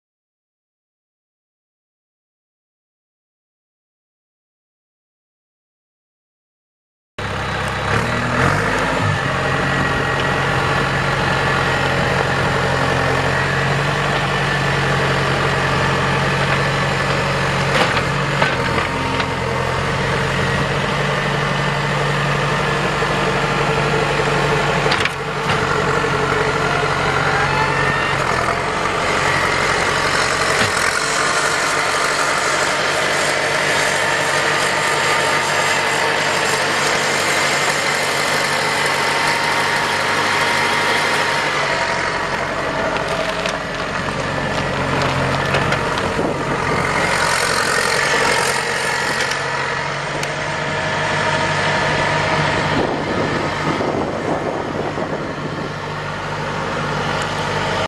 2003 Case 90XT skid steer for sale | sold at auction August 13, 2015
2003 Case 90XT skid steer
1,771 hours on meter
Case 4T-390 3.9L four cylinder turbo diesel engine
85 HP
Two speed hydrostatic transmission
Enclosed cab
Heat
EMS panel
Joystick controls
Auxiliary hydraulics
Front work light
12-16.5 NHS tires
8,950 lbs. base operating weight
Cab door is not included
Serial JAF0392638

It was located in Centerville, IA, and we sold it to the highest bidder for $12,650.